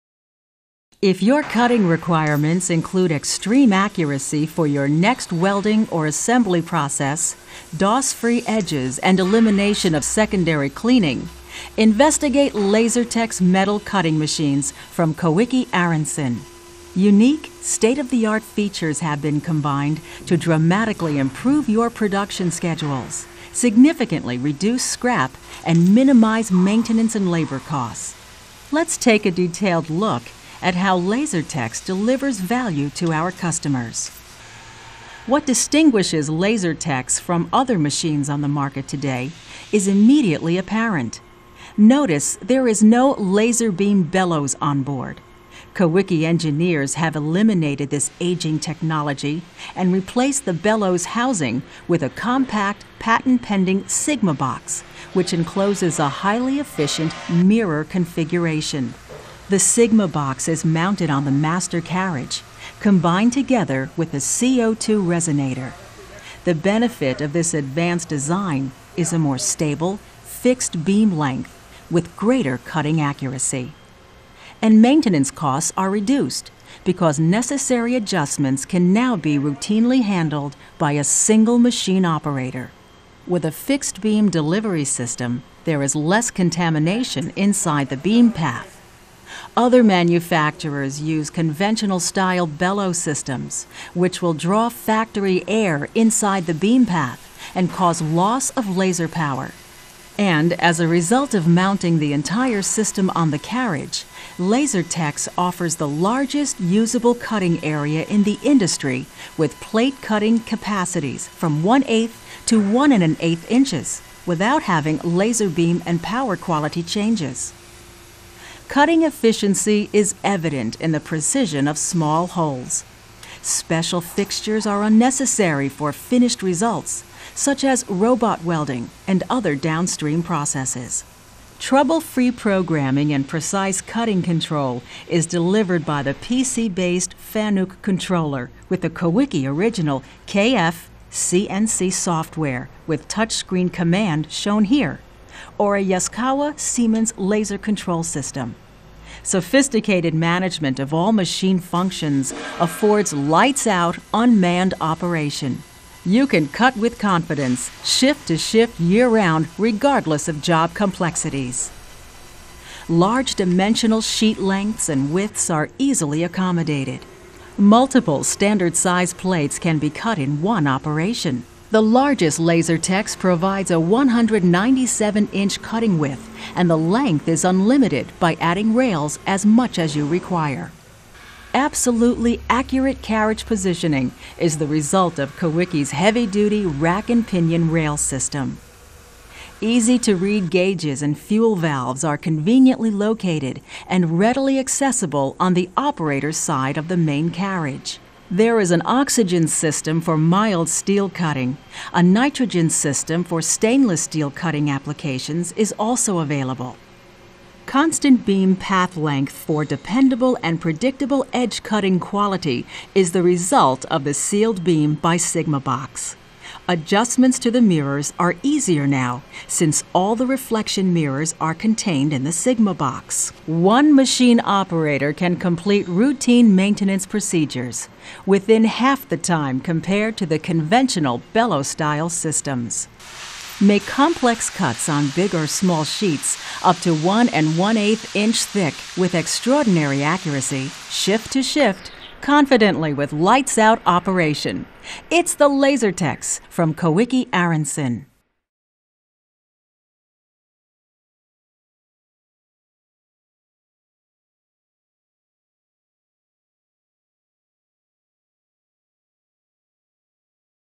Extreme Accuracy Lasertex demonstration video
If your cutting requirements include extreme accuracy for your next welding or assembly process, doss free edges, and elimination of secondary cleaning, investigate Lasertex metal cutting machines from Koike Aronson.
Unique state-of-the-art features have been combined to dramatically improve your production schedules. Significantly reduce scrap and minimize maintenance and labor costs. Let's take a detailed look at how Lastertex delivers values to our customers.
What distinguishes Lastertex from other machines in the market today is immediately apparent. Notice, there is no laser beam bellows on board. Koike engineers have eliminated this aging technology and replaced the bellows housing with a compact, patent pending sigma box; which encloses a highly efficient mirror configuration. The sigma box is mounted on the master carriage, combined together with the CO2 resonator. The benefit of this advanced design is a more stable fixed beam length with greater cutting accuracy. And maintenance costs are reduced because necessary adjustment can now be routinely handled by a single machine operator.
With a fixed beam delivery system there is less contamination inside the beam path. Other manufacturers use conventional style bellow systems which will draw factory air inside the beam path and cause loss of laser power. And as a result of mounting the entire system on the carriage, Lasertex offers the largest usable cutting area in the industry. With plate cutting capacities from 1/8th to 1 1/8 inches, without having laser beam and power quality changes. Cutting efficiency is evident in the precision of small holes. Special fixtures are unnecessary for finished results, such as robot welding and other down string processes.
Trouble free programing and precise cutting control is delivered by the PC based controller with the Koike original CNC software, with touch screen command shown here. Or a laser control system. Sophisticated management of all machine functions affords lights out, unmanned operation. You can cut with confidence shift to shift, year-round, regardless of job complexities. Large dimensional sheet lengths and widths are easily accommodated.  Multiple standard sized plates can be cut in one operation. The largest Lasertex provides a 197 inch cutting width, and the length is unlimited by adding rails as much as you require. Absolute accurate carriage positioning is the result of Koike's heavy duty, rack and pinion rail system. Easy to read gauges and fuel valves are conveniently located and readily accessible on the operator side of the main carriage. There is an oxygen system for mild steel cutting; a nitrogen system for stainless steel cutting applications is also available. Constant beam path length for dependable and predicable edge cutting quality is the result of the sealed beam sigma box. Adjustments to the mirrors are easier now since all the reflection mirrors are contained in the sigma box. One machine operator can complete route maintenance procedures within half the time compared to the conventional bellow style systems. Make complex cuts on big or small sheets up to 1 1/8 inch thick with extraordinary accuracy shift to shift confidently with lights out operation. It's the Lastertex from Koike Aronson.